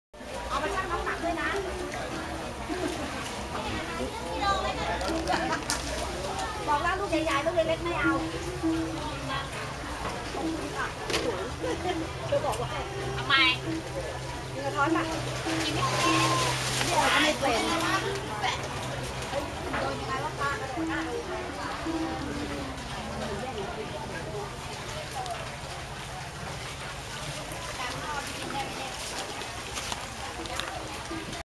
タイ タリンチャン水上マーケットの様子・・・3・魚類の店
タイ 　バンコク近郊　タリンチャン水上マーケットの様子・・・3
魚類の店・・・ペット・食用？